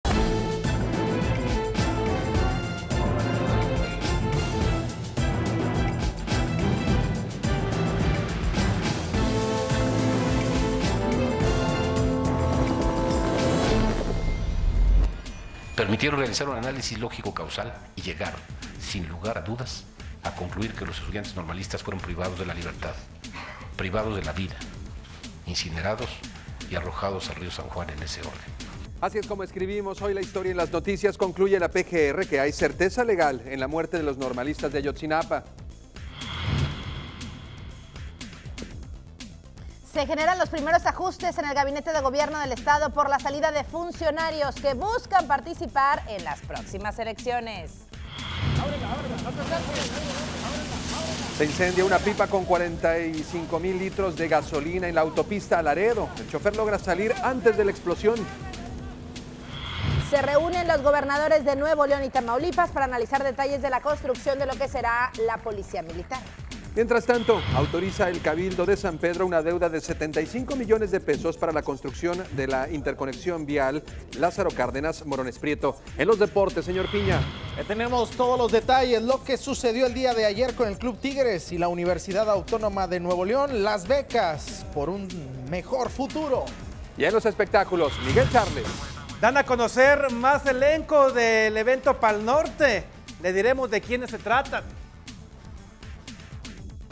Las Noticias - Los titulares del miércoles 28 de enero de 2015
Normalistas fueron asesinados.- PGR;ajustes en gobierno del estado;incendio de pipa con 45 mil litros de gasolina; se reunen gobernadores de NL y Tamaulipas; autoriza cabildo de San Pedro deuda para construcción; Tigres por un futuro mejor; mas elenco de Pal Norte